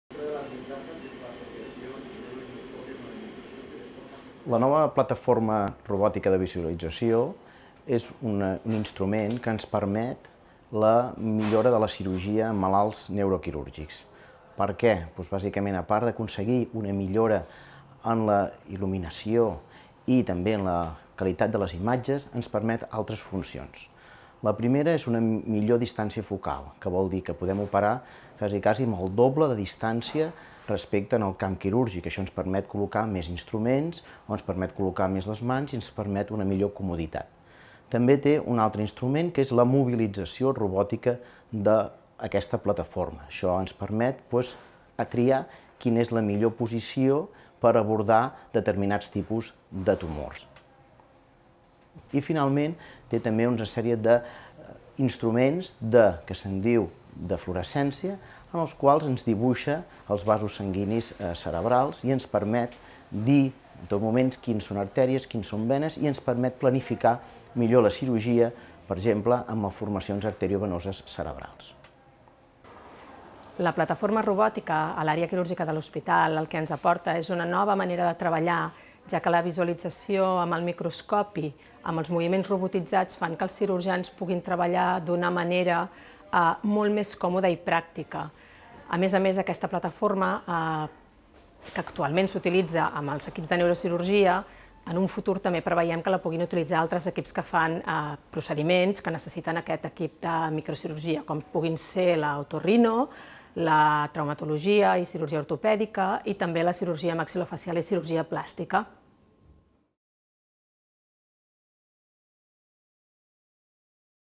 Nova plataforma robòtica de visualització per a intervencions en neurocirurgia
L’Hospital Clínic compta amb una nova plataforma robòtica de visualització d’última generació. Es tracta d’un microscopi quirúrgic que incorpora diferents funcionalitats que permeten una millor visió de la zona del cervell que s’opera i una millor navegació per tenir localitzades les lesions o zones on es practica la cirurgia.